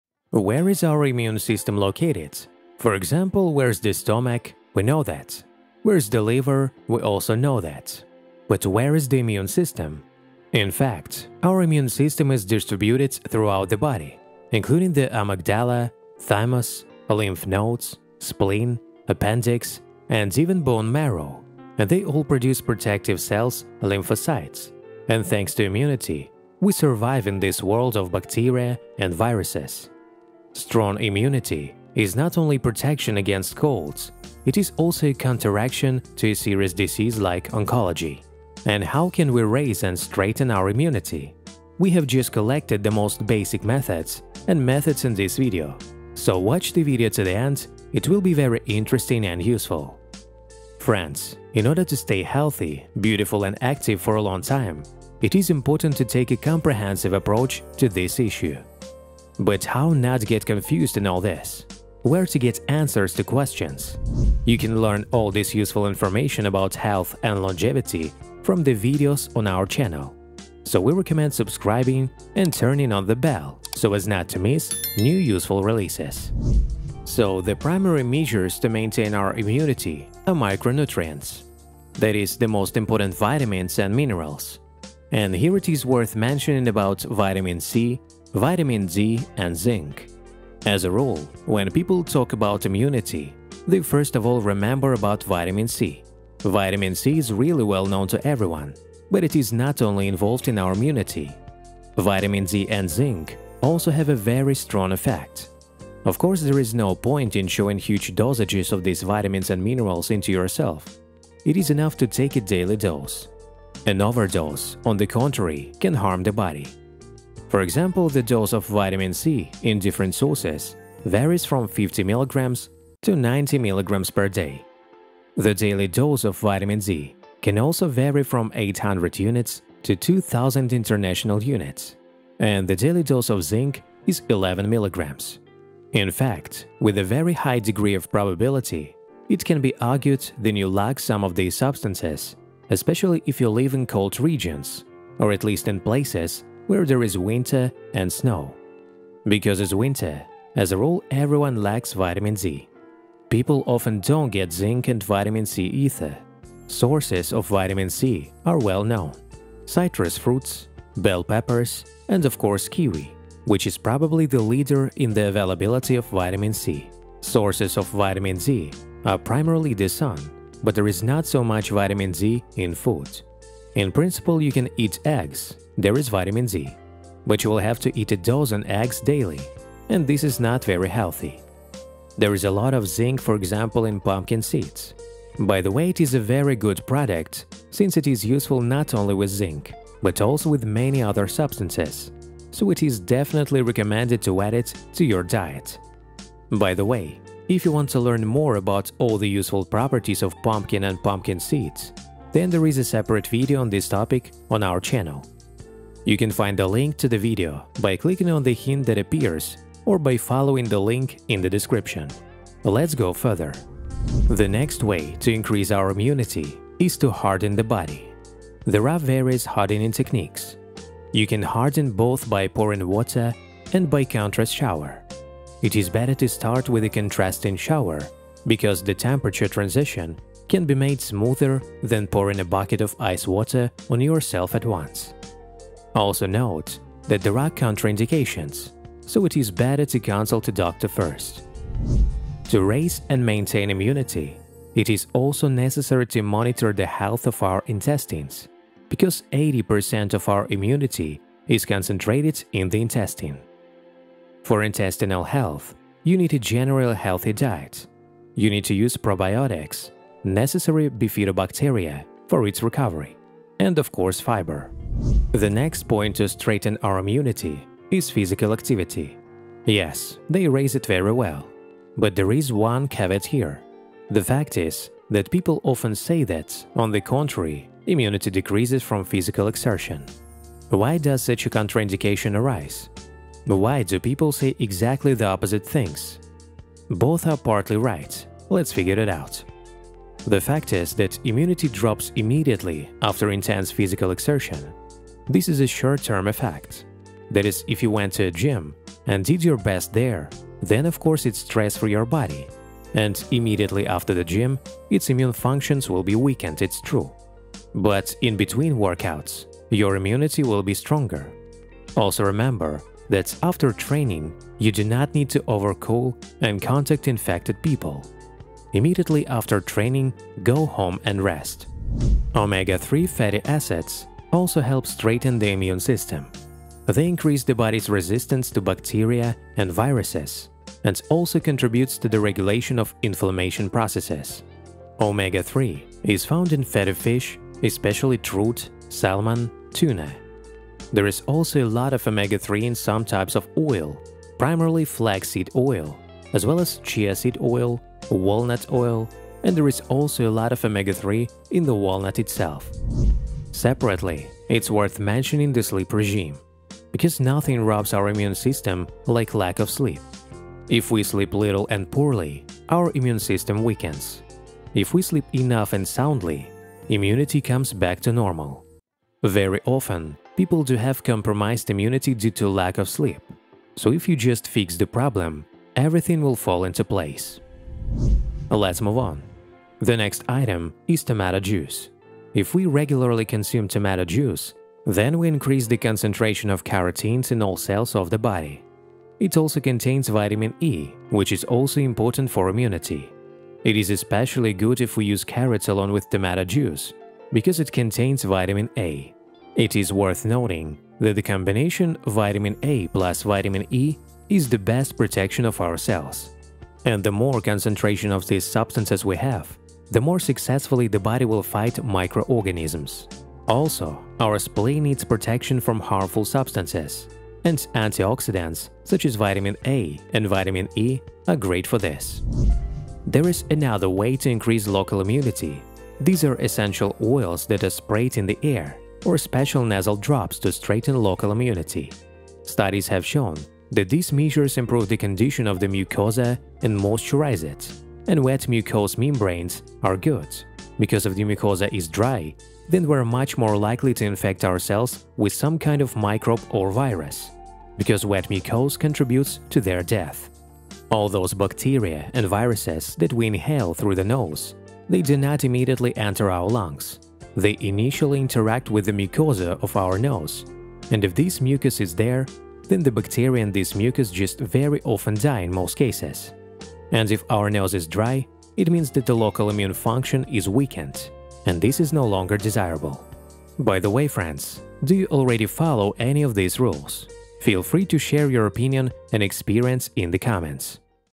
I Haven't Been Sick in 35 Years! Here are My Secrets to Eternal Immunity!
Where is our immunity? For example, where is the stomach - we know that, where is the liver - we also know that, but where is the immune system? In fact, our immune system is distributed throughout the body, it is the amygdala, thymus, lymph nodes, spleen, appendix, and even bone marrow, and they all produce protective cells - lymphocytes, and thanks to immunity we survive in this world of bacteria and viruses. Strong immunity is not only protection against colds, it is also a counteraction to a serious disease like oncology. And how can we raise and strengthen our immunity? We have just collected the most basic methods and methods in this video. So watch the video to the end, it will be very interesting and useful.